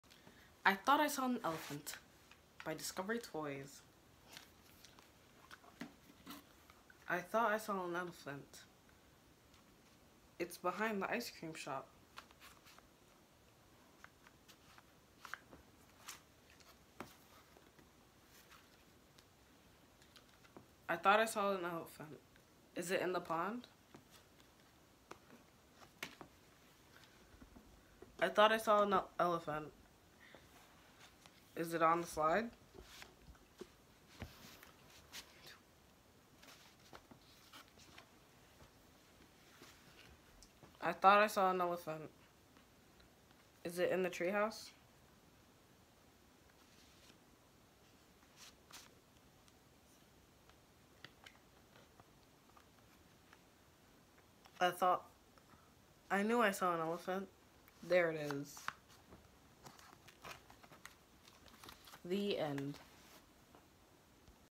I thought I saw an Elephant 🐘
Grandson reading a book 📖 sold by Discovery Toys.